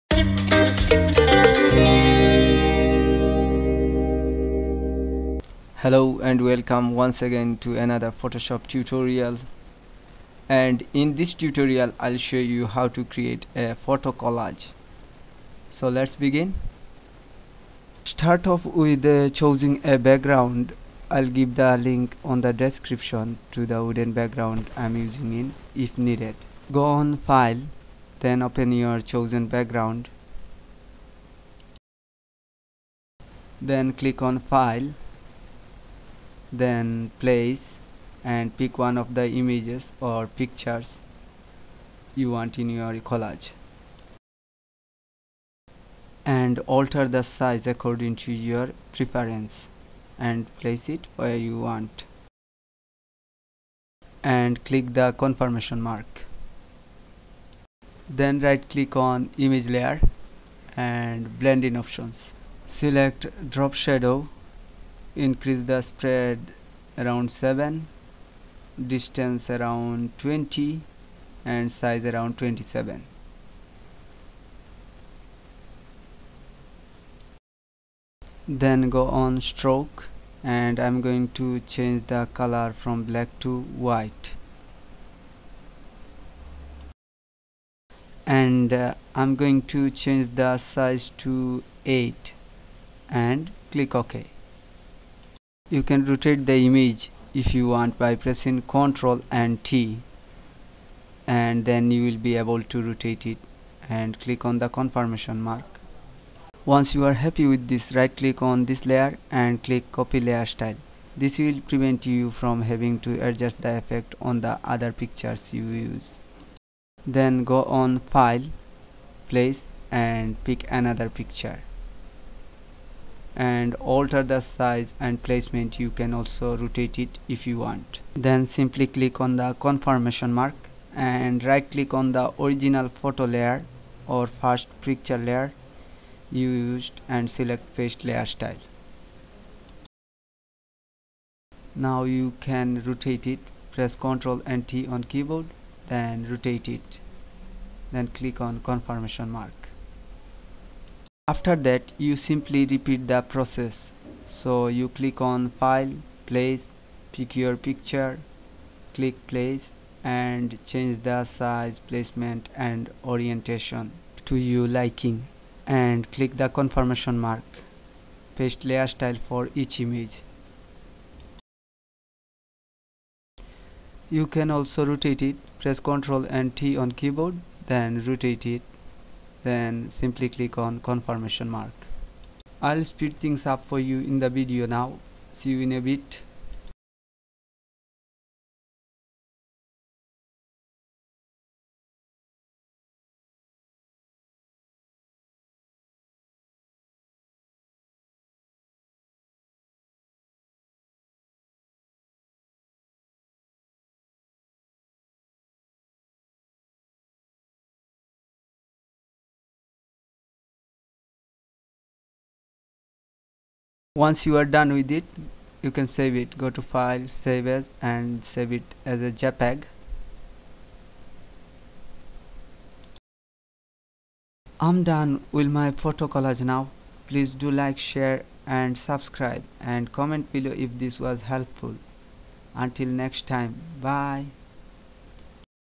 How to Create Photo Collage in Photoshop
This is Photo collage tutorial in Photoshop, this video is for Photoshop beginners, if you are looking to put multiple number image in one document you can follow this video and it would be easier for you to understand How to Make Photo collage  in Photoshop and you could apply it on your Images,it is easy for anyone